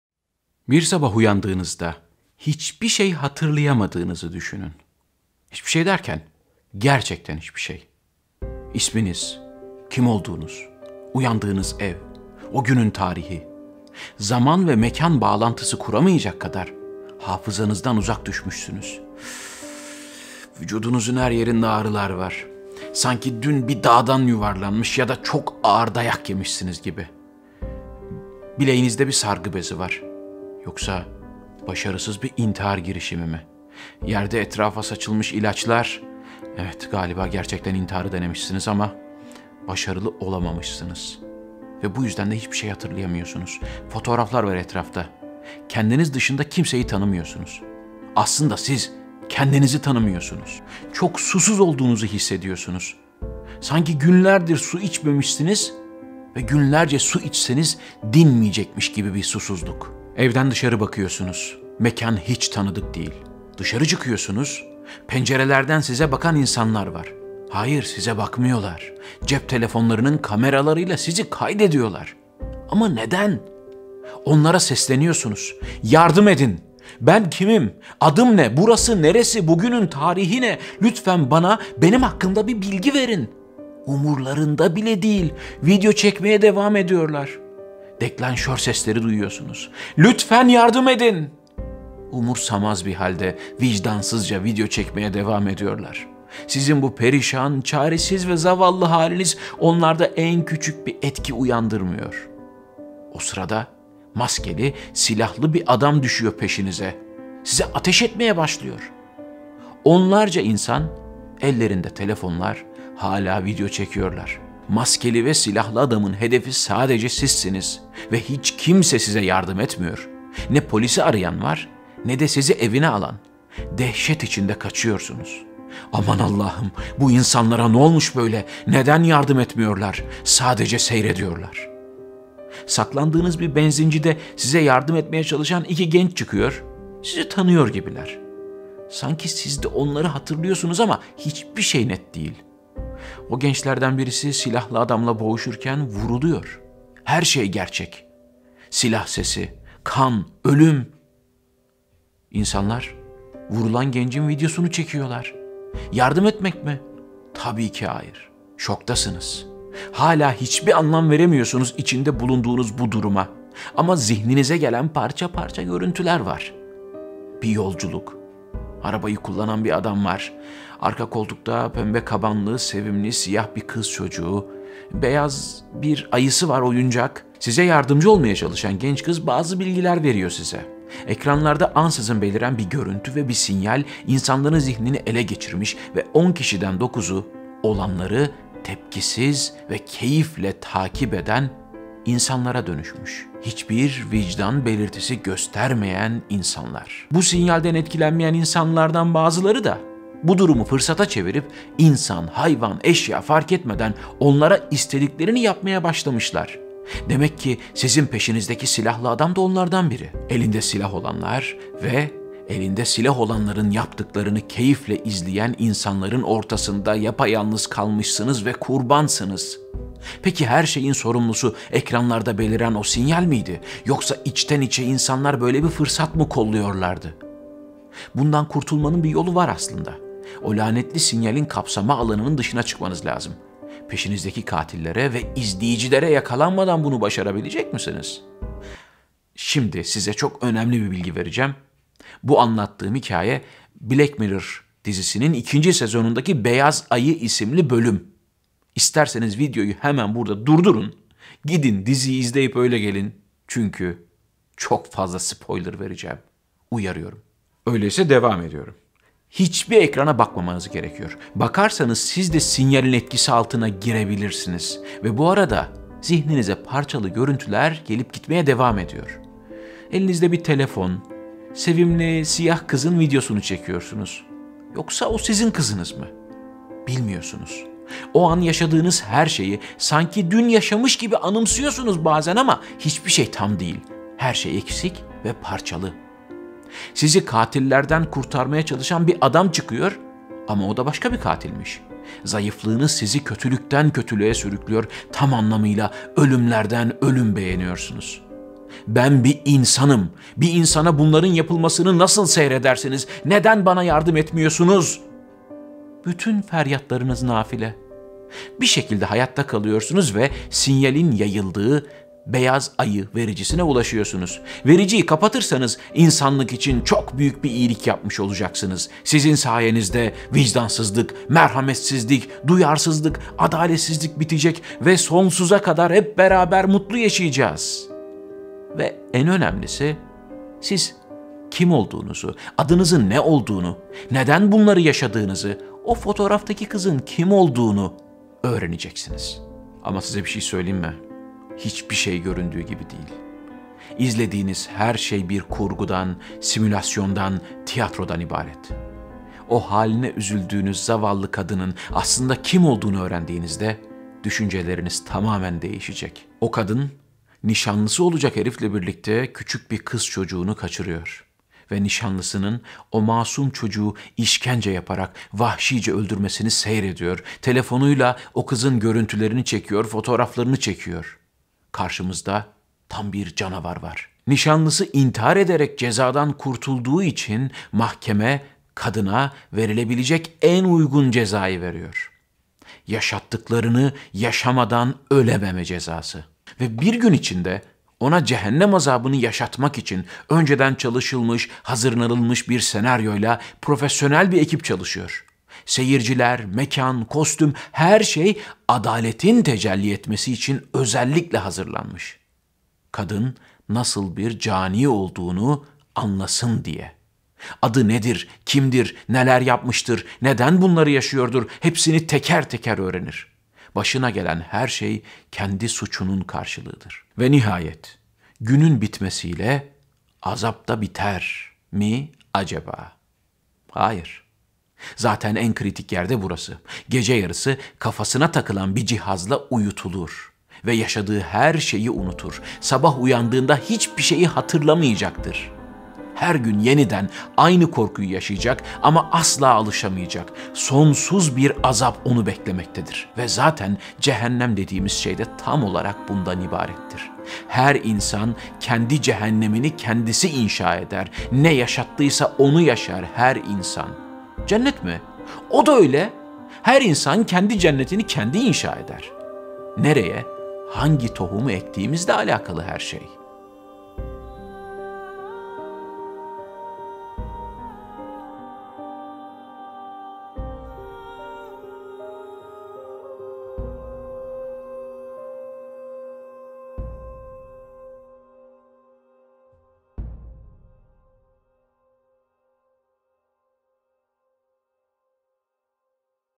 CEHENNEM NASIL BİR YER? | ANLATACAKLARIM VAR
Bir sabah uyandığınızda hiçbir şey hatırlayamadığınızı düşünün.
Hiçbir şey derken, gerçekten hiçbir şey.
İsminizi, kim olduğunuzu, uyandığınız evi, o günün tarihini… Zaman ve mekân bağlantısı kuramayacak kadar hafızanıza erişim sağlayamıyorsunuz.
Vücudunuzun her yerinde ağrılar var. Sanki dün bir dağdan yuvarlanmış ya da çok ağır dayak yemişsiniz gibi…

Sizce cehennem nasıl bir yerdir?
Cehennem azabı nasıl bir şeydir?